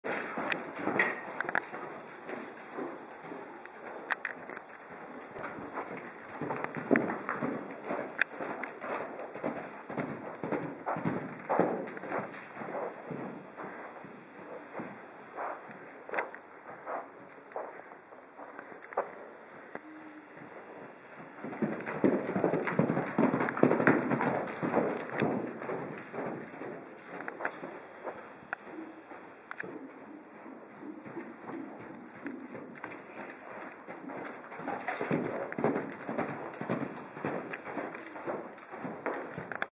hytyme Atlas Moving 6-4-13
Atlas showing his power and grace in various movements in the equine world.